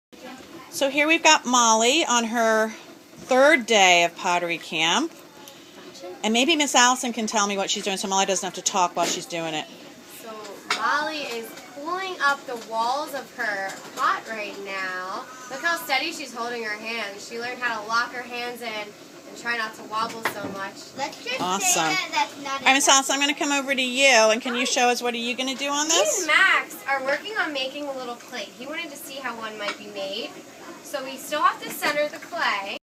Molly at the pottery wheel
Molly demonstrates how to pull up the walls of her pottery piece on the pottery wheel at Pottery Specialty Camp at The Center on Central in Paoli, PA